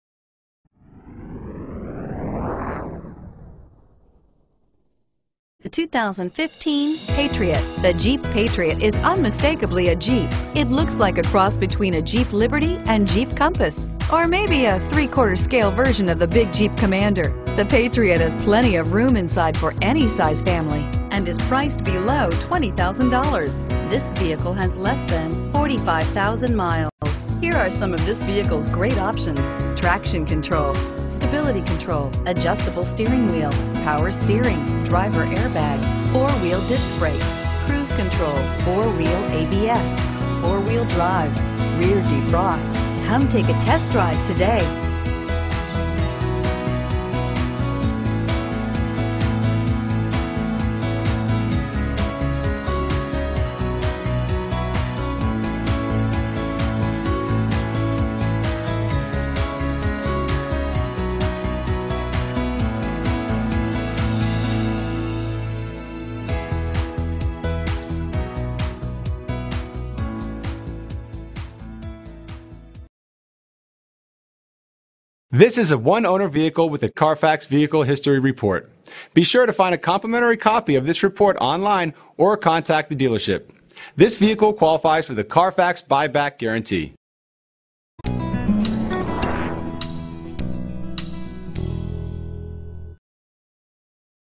2015 Jeep Patriot Alcoa TN 214033
2015 Jeep Patriot Sport 4WD

West Chevrolet
3450 Airport Highway

Looking for the right ? Today could be your lucky day. With 40402 miles, this Silver, 2015 Jeep Patriot equipped with Automatic transmission could be yours. Request more information and set up a test drive right away.